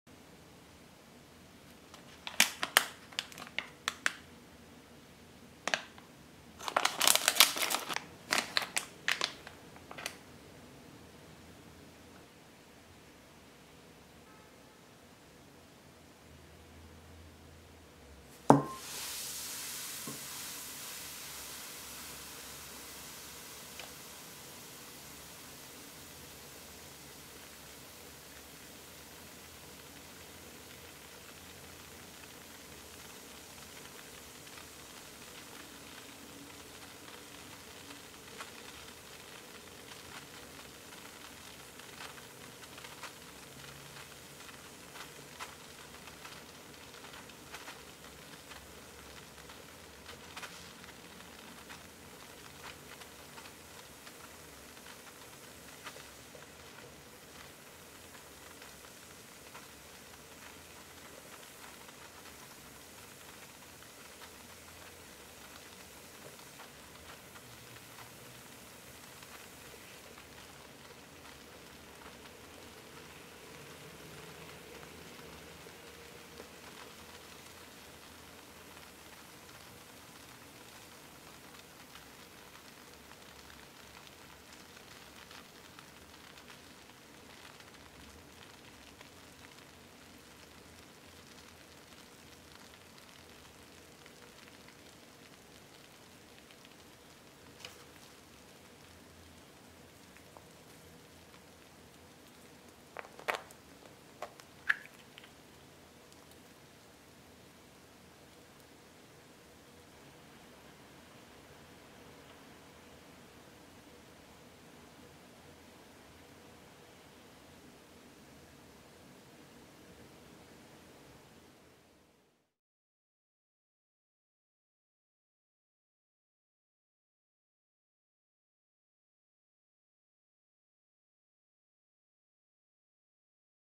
ASMR Prize Bath Bomb #204 Ultraman
It has a grapefruit scent.
I like the fizzy sound and the prize.